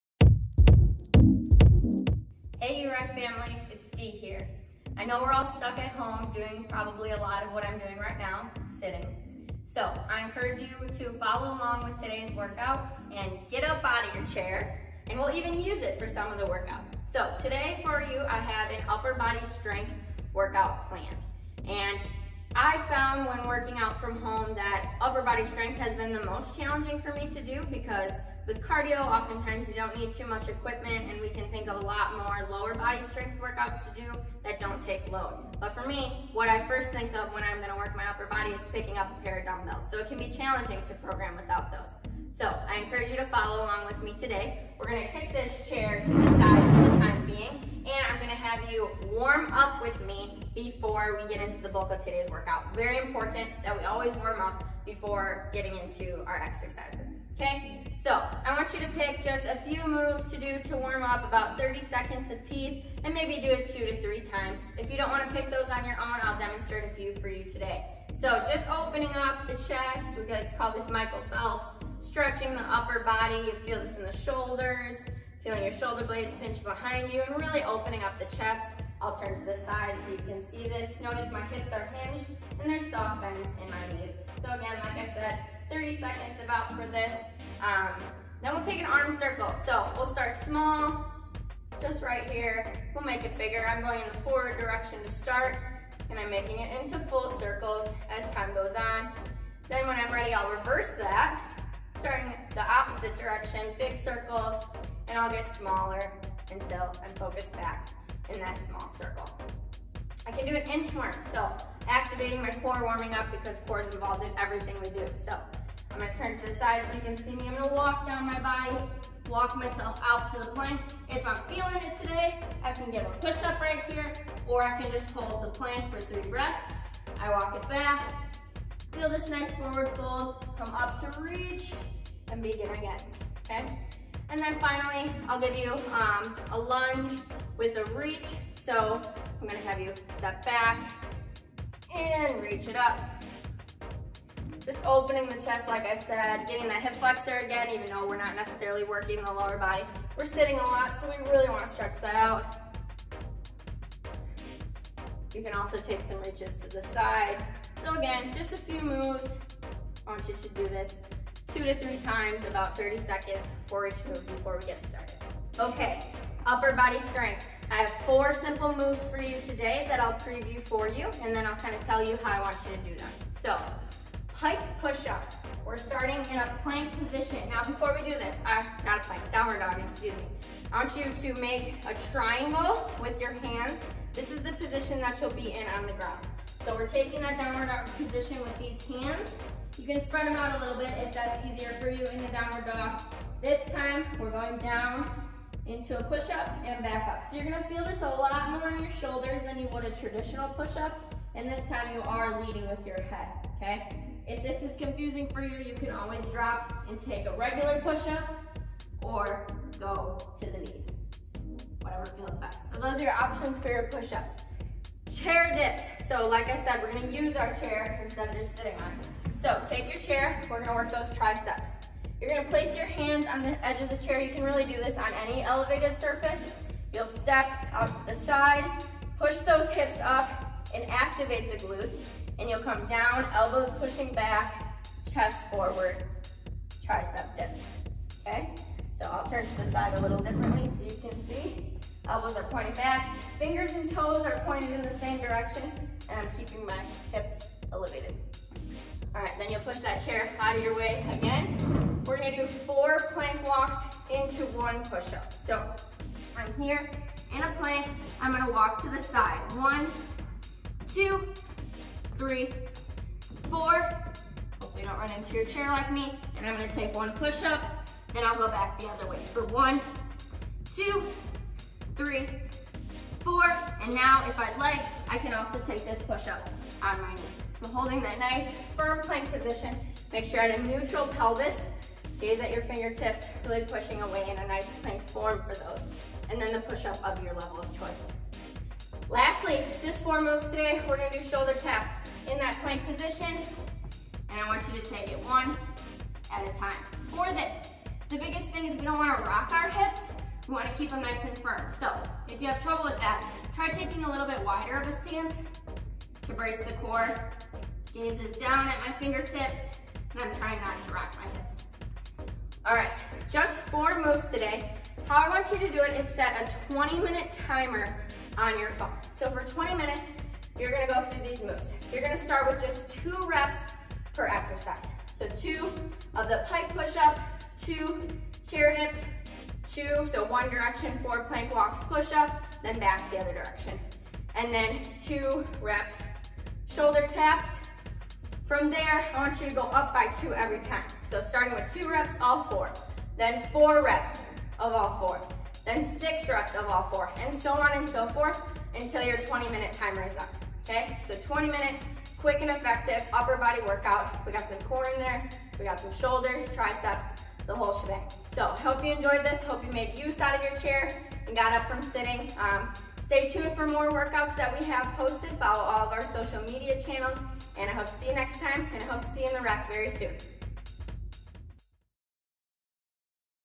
Upper Body Strength
We will engage your arms, back, and core in this bodyweight upper body strength workout.